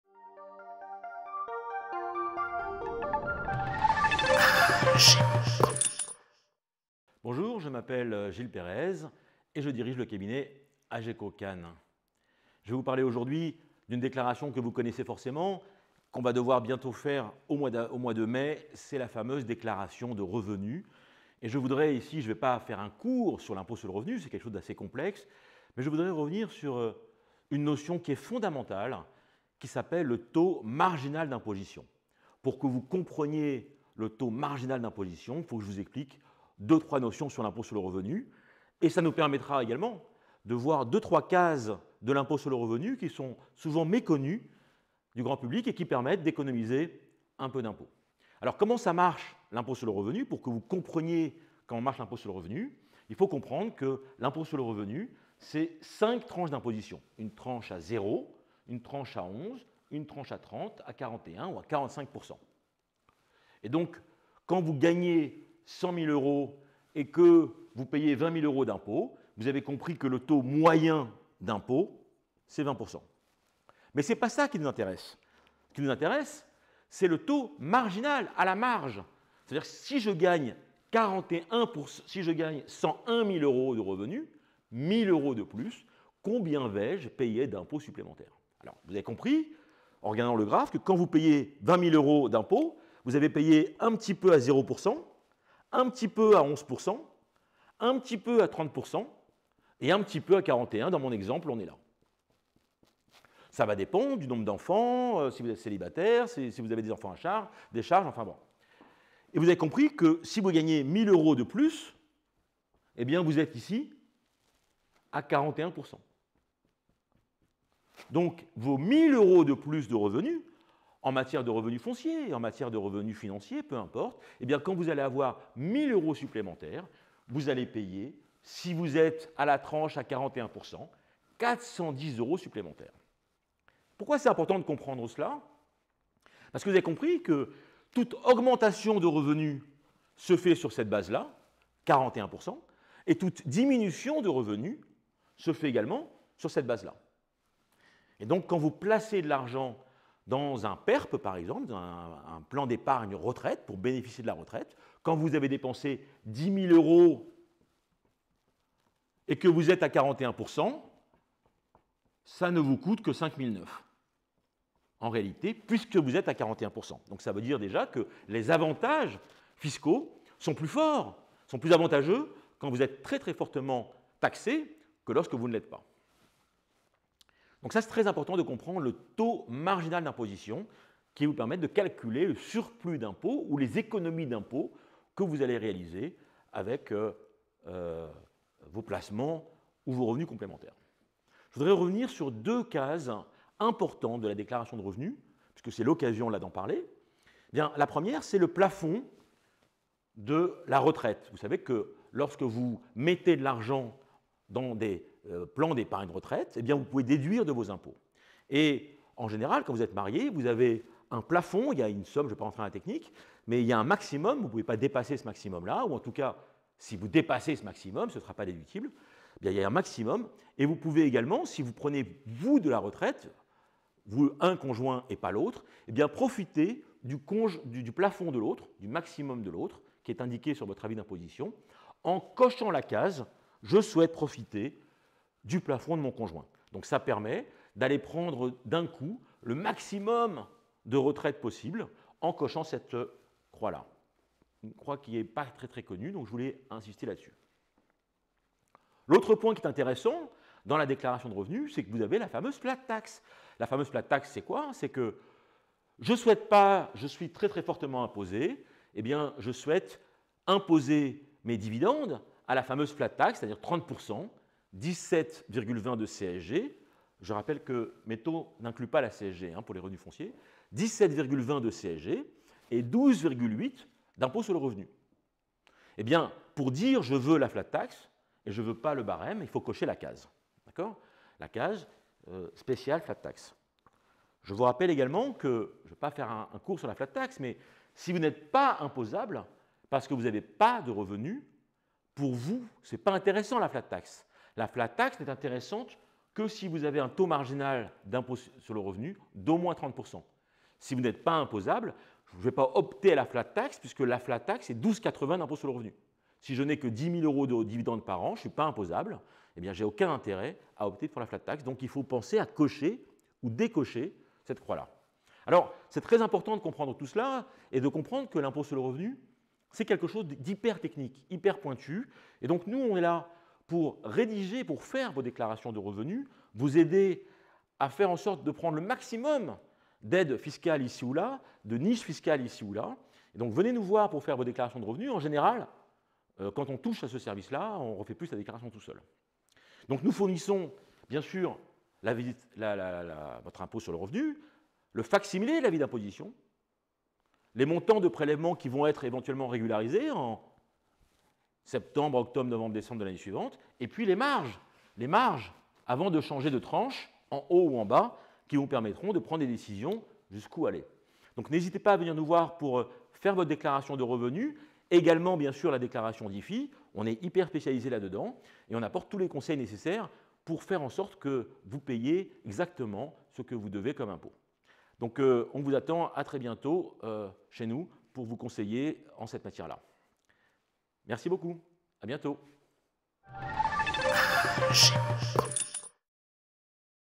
Le taux marginal d'imposition
Comprenez rapidement le taux marginal d'imposition. Le fameux TMI !